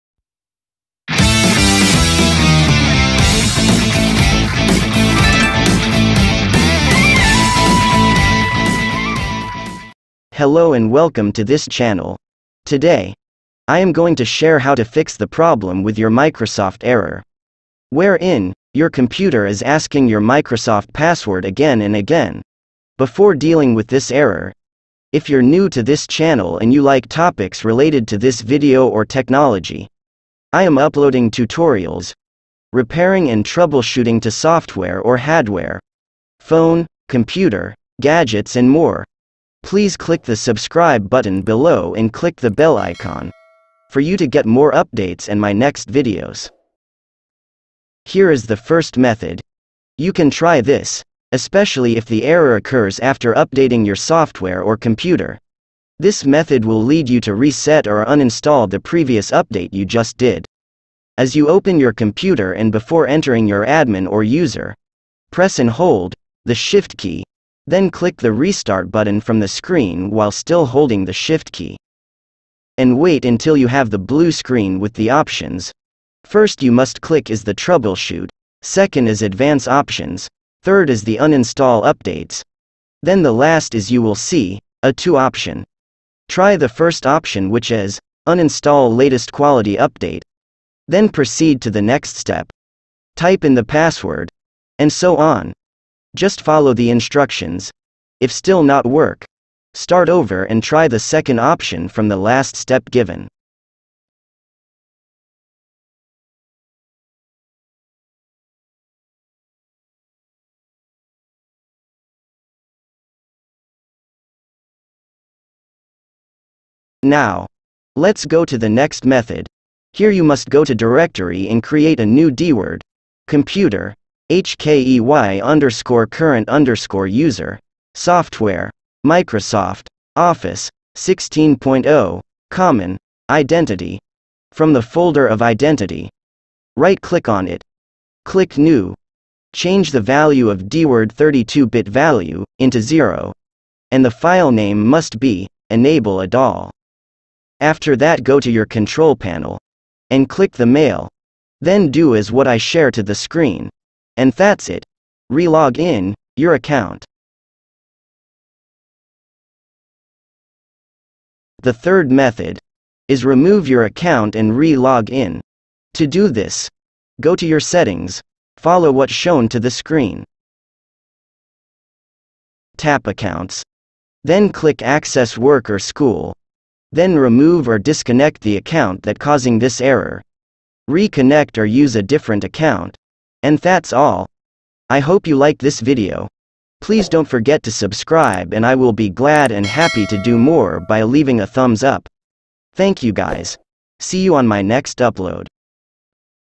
Repeatedly asking password or Showing wrong password (3 Method to fix)
Having trouble with your microsoft account or software? This video will show you how to fix this error.

Content:

00:47 - 01:44 "1st Method - Uninstalling Updates"
01:53 - 02:39 "2nd Method - Modify Software/ Creating new value"
02:40 - 03:15 "Disconnecting account/ Relog in or Use different account"